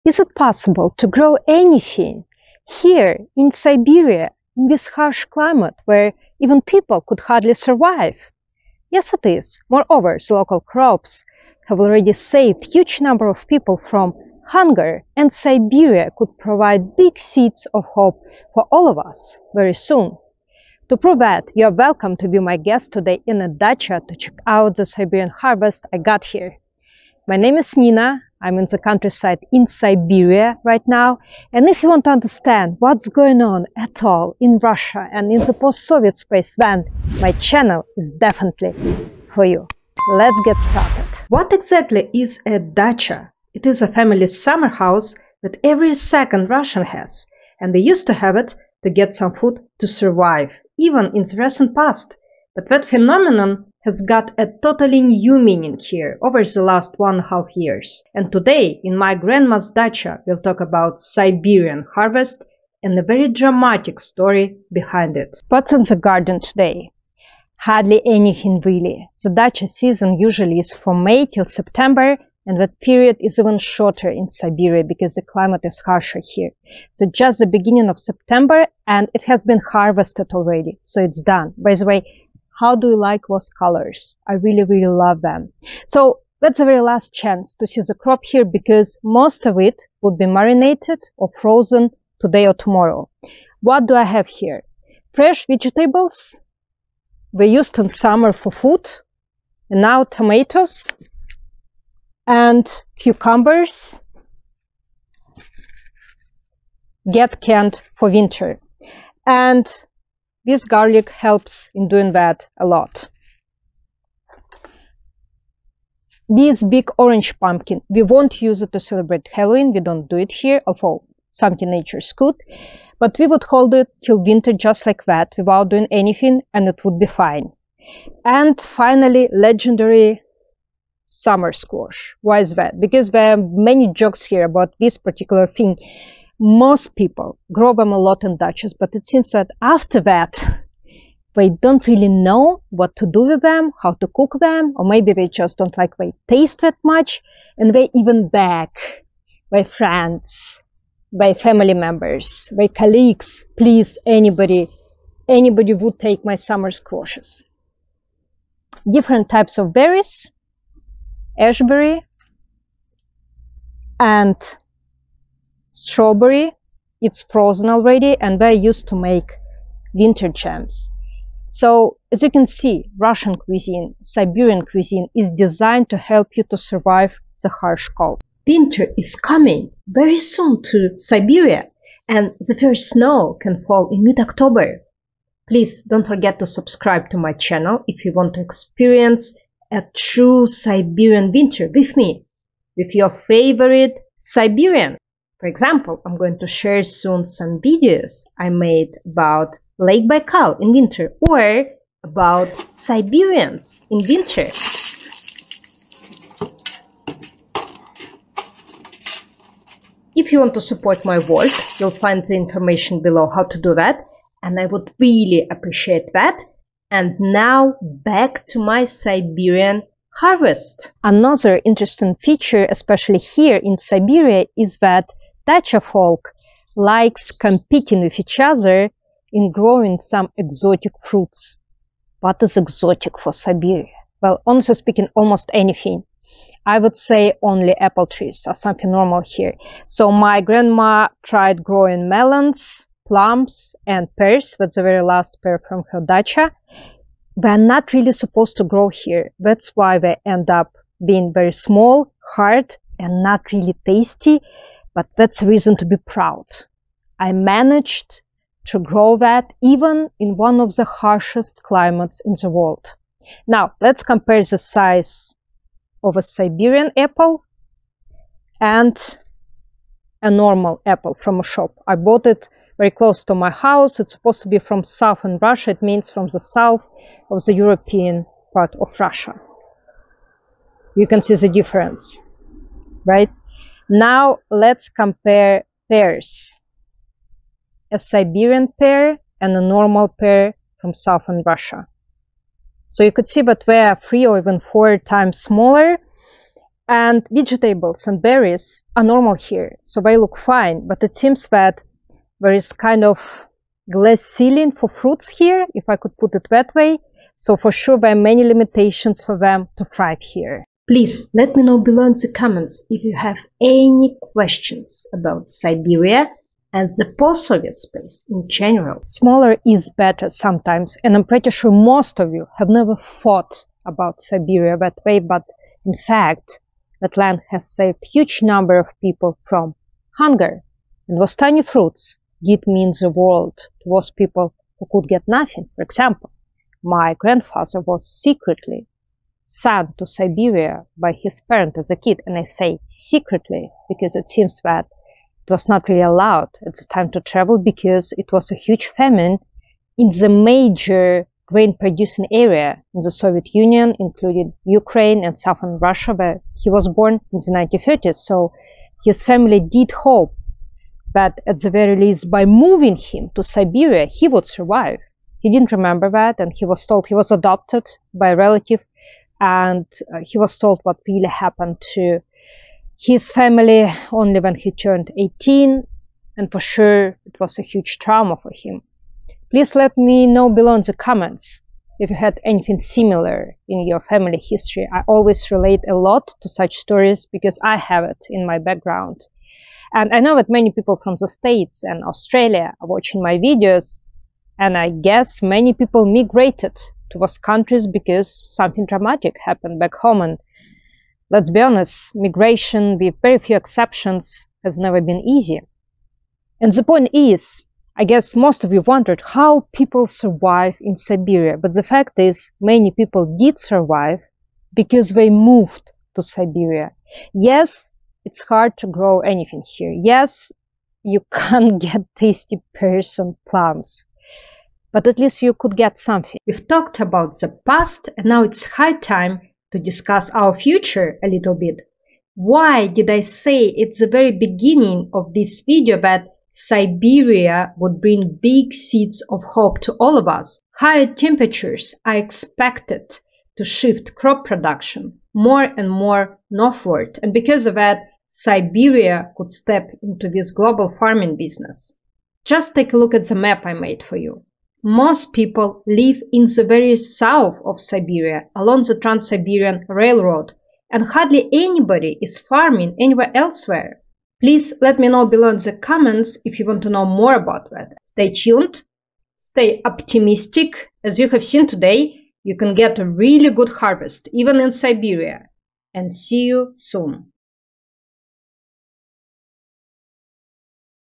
Surviving Siberia: Harvest in Extreme Conditions How Siberians live and farm
How to survive in Siberia? How to grow crops and vegetables in this harsh extreme climate? How to get exotic and rare plants and fruits? Is this harvesting and farming sustainable? What are the most common challenges that local farmers face? Local crops have already saved huge number of people and Siberia could provide big seeds of hope for all of us soon. In this video I'll show you what Siberians do in the countryside, how they engage with nature and make the most out of agriculture. Today you’re welcomed to check out my Siberian harvest I got here in a farm or dacha.
Time codes:
00:01 In my farm in Siberia
00:41 What is a dacha?
01:05 My harvest
03:40 Siberian melons, plums and pears
05:19 How people survive in Siberia
07:07 Seeds of hope